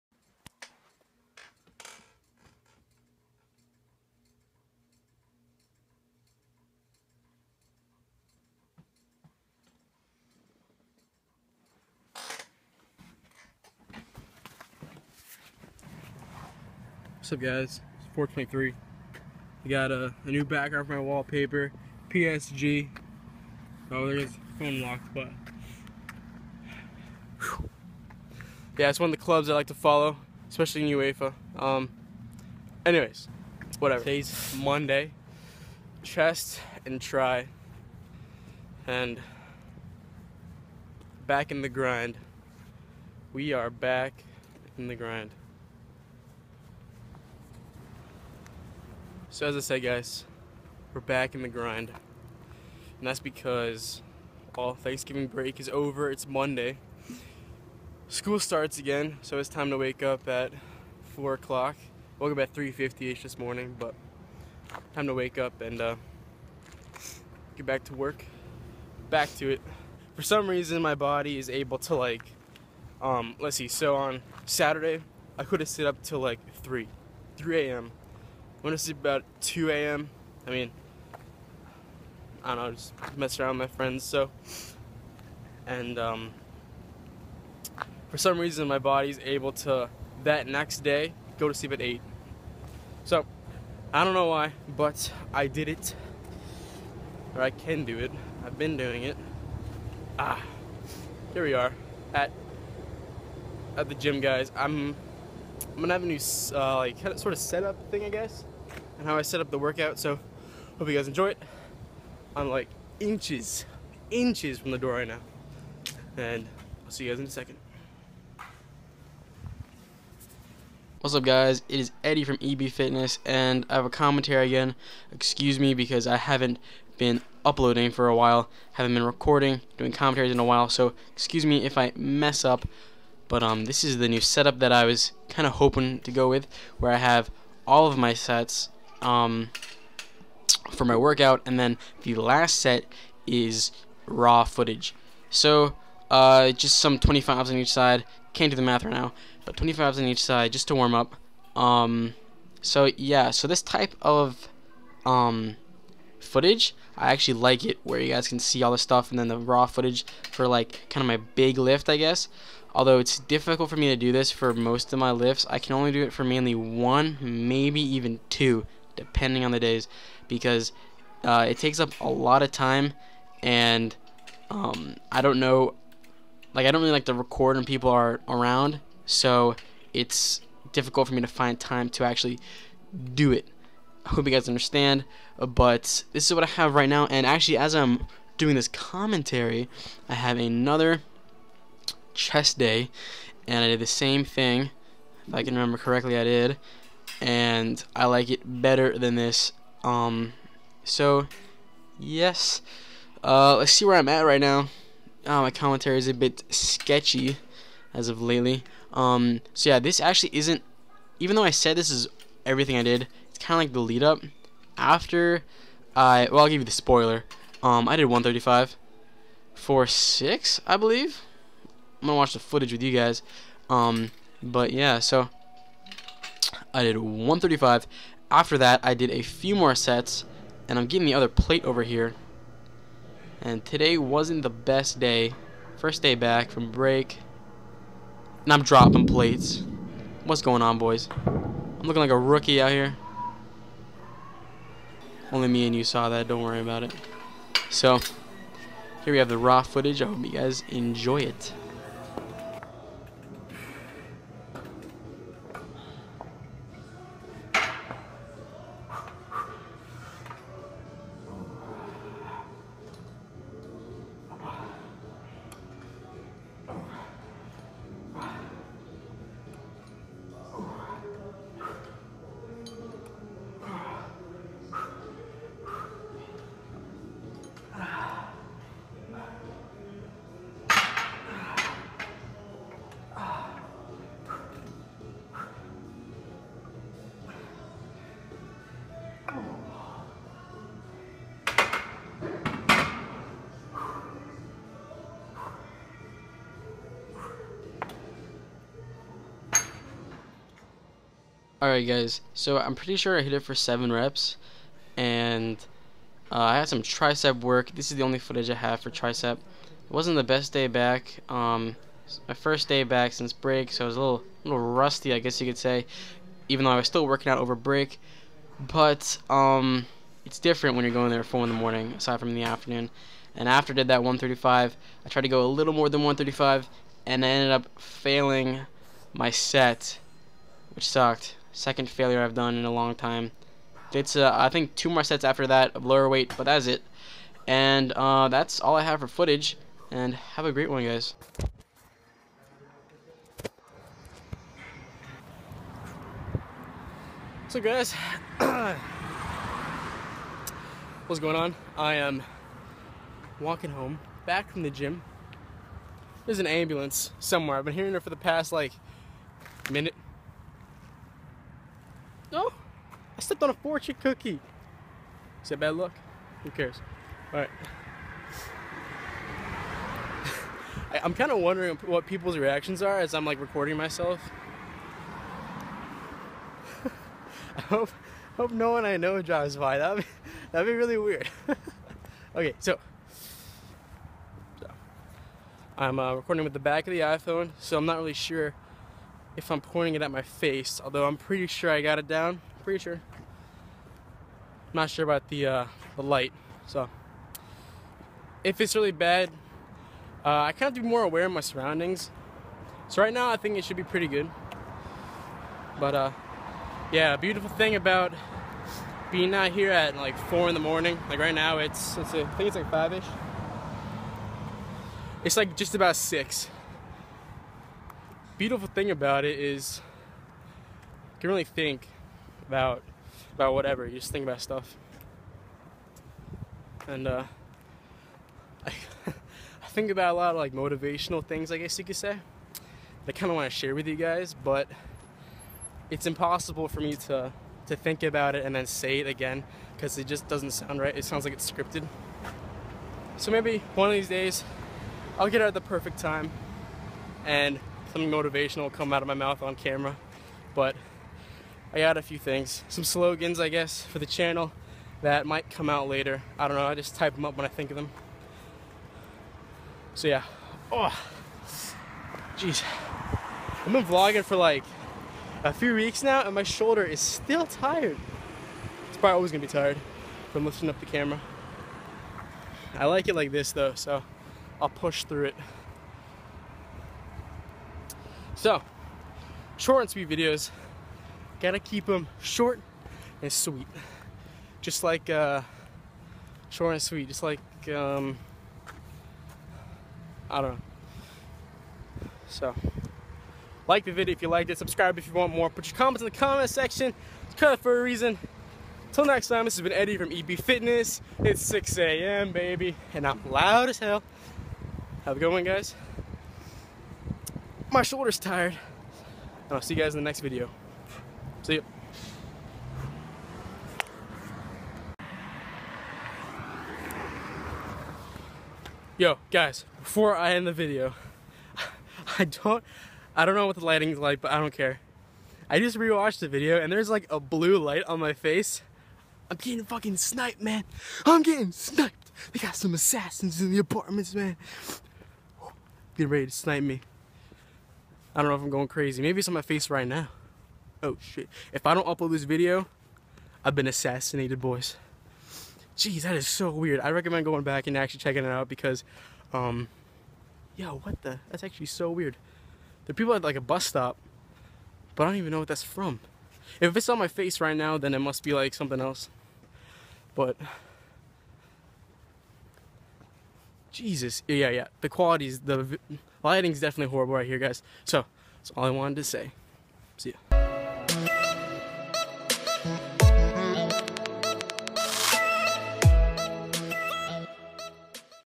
Back to the Grind Boys
We're back to waking up at 4am! It's really different lifting at 4am, then at 1 in the afternoon. Maybe it's more of a mental thing.. I'm not sure...